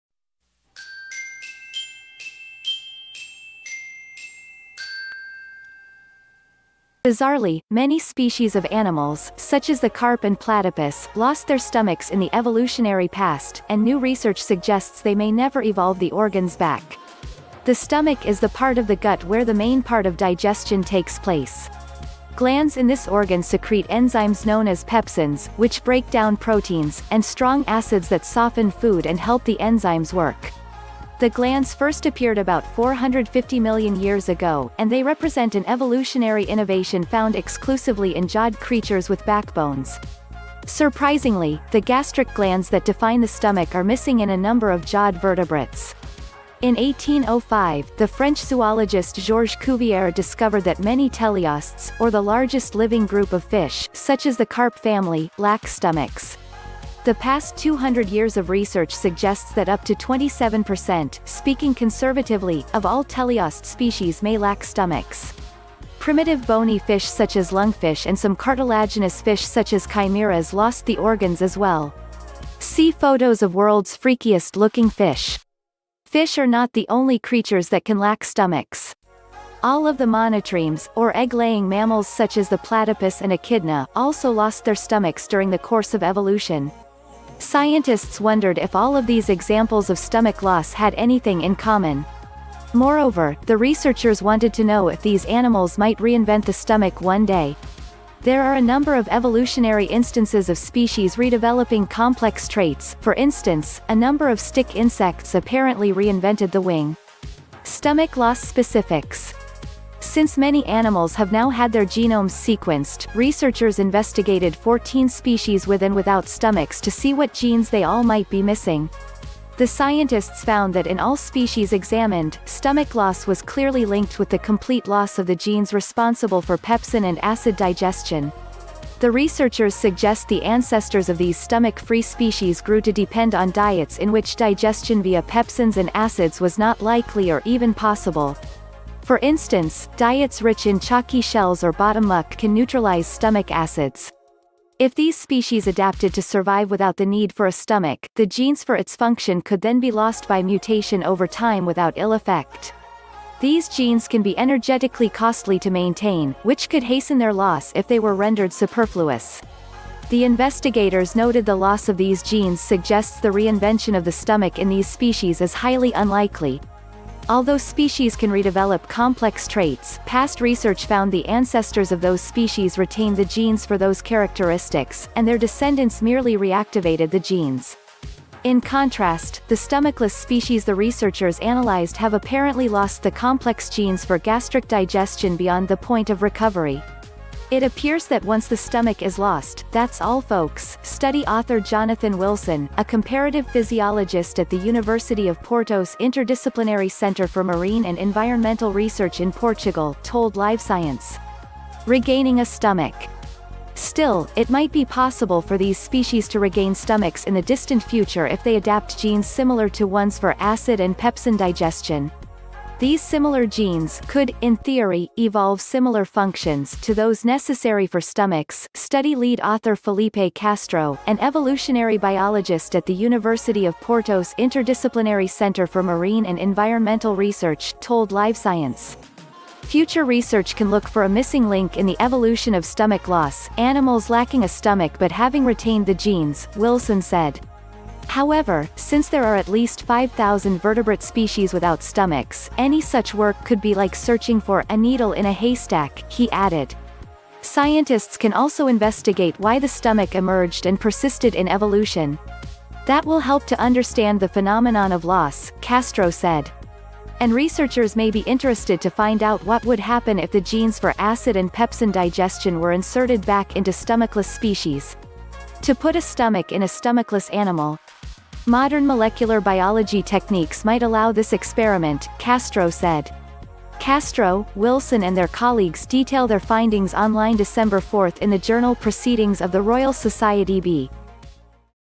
What! Platypus don't have stomach.
Yeah it's true platypus don't have stomach but if you don't know why then it is not good because
'Half knowledge is harmful'